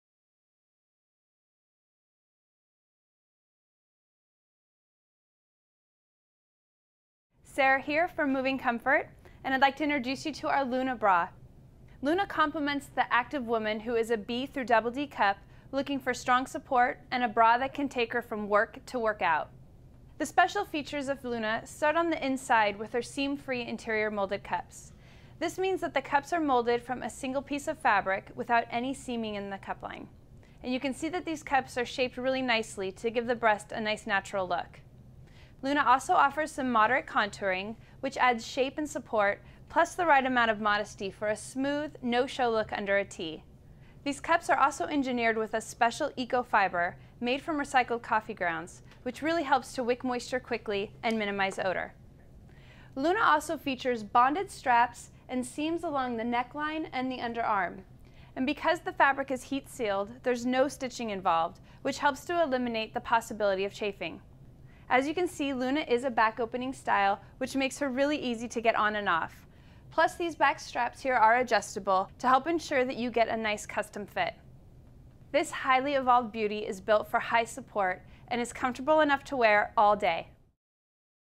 Luna Sports Bra by Moving Comfort
The Luna sports bra is built for maximum support and comfort. Moderate contour cups add shape and a smooth, no-show look under a tee. Watch the video to learn what makes her special, and tips for getting the perfect fit.